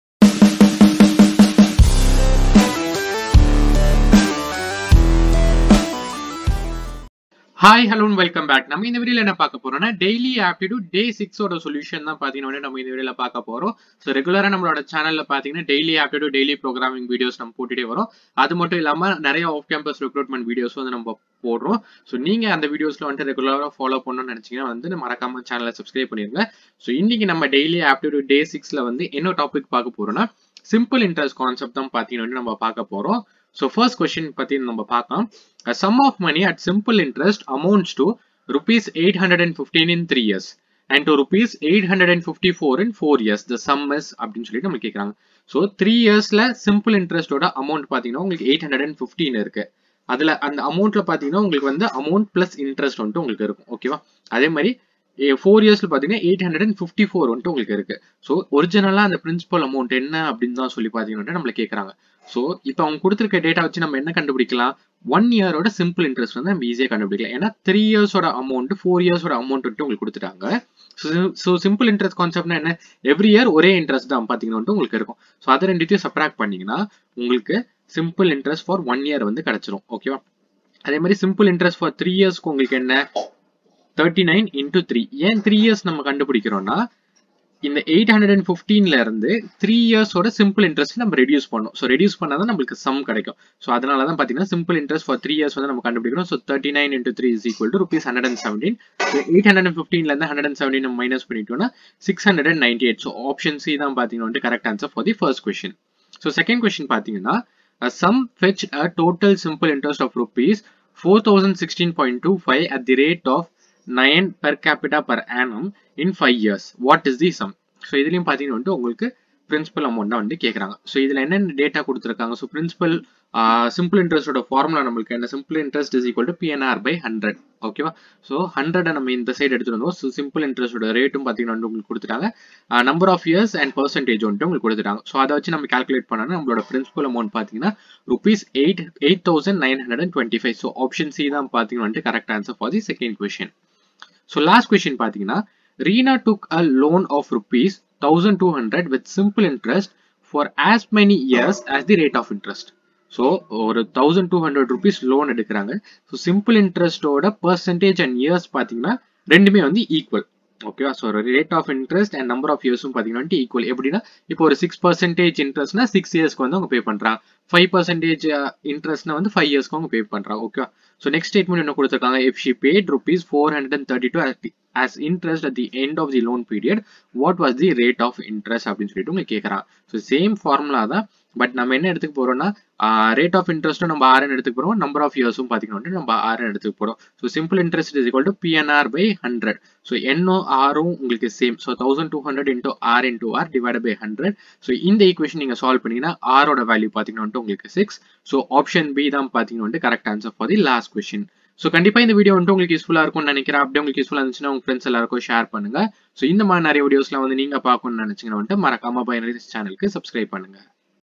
Simple Interest Aptitude Questions | Daily Aptitude - Day 6 Solution | BiNaRiEs
In this video, I will be discussing with you about the Placement Aptitude Preparation Plan.